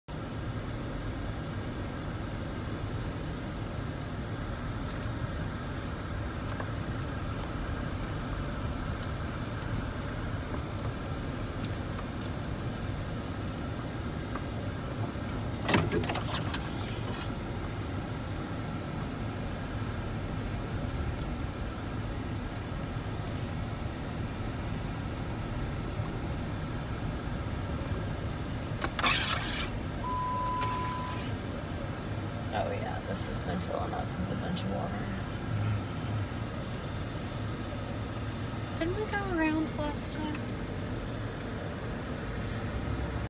Where I got my name
Here’s the area where I got the name “StuckInMudHole”